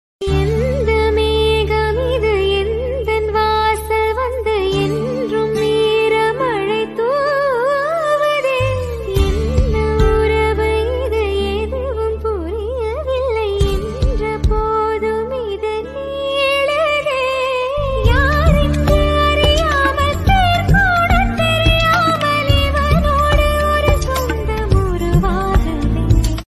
இது ஒரு முறை செஞ்சு பாருங்க சுவையான உரைப்பு அடை tasty crispy adai recipe🤤😋😍 night dinner recipe🤤😍😋😋
ingredients

for soak
rice 1 cup
urad dal 1 hand full
channa dal 1 hand full
thoor dal 1 hand full
 and grind

fennel seed powder
coconut
salt
chilli powder
onion chopped
green chilli chopped
curry leaves chopped
ginger chopped

and also grind